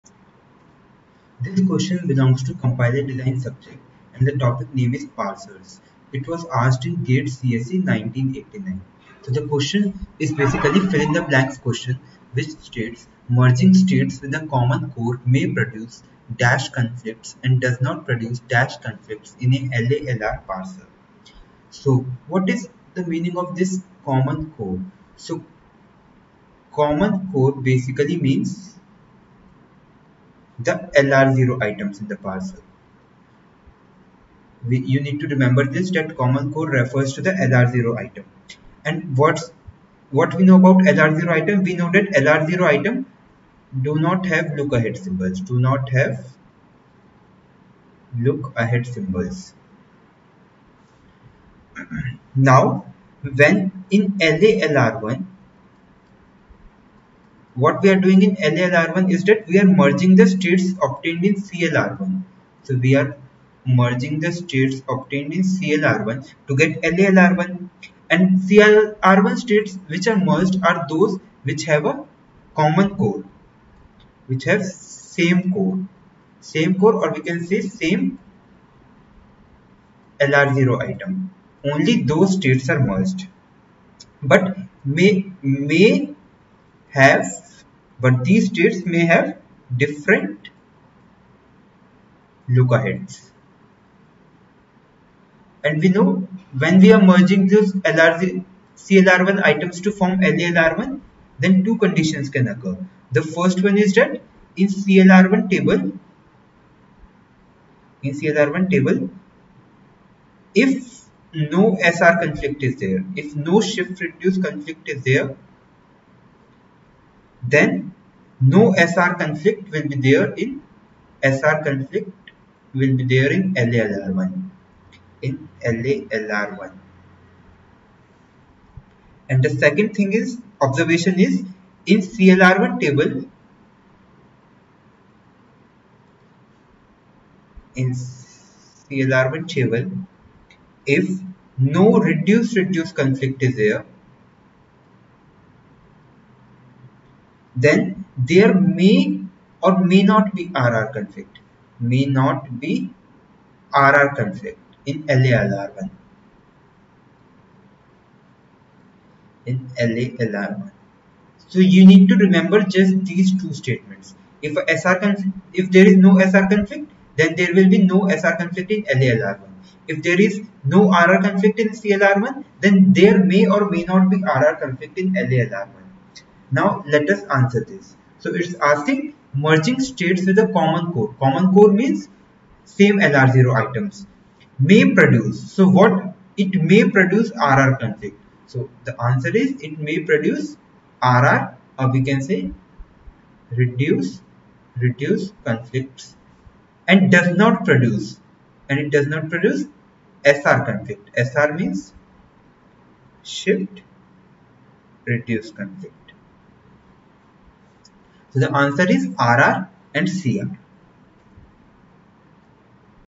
GATE 1989 | CD | LALR PARSER | CONFLICT | GATE TEST SERIES | SOLUTIONS ADDA | EXPLAINED BY VIVEK
GATE 1989 Q3: Merging states with a common core may produce __________ conflicts and does not produce ___________ conflicts in an LALR purser.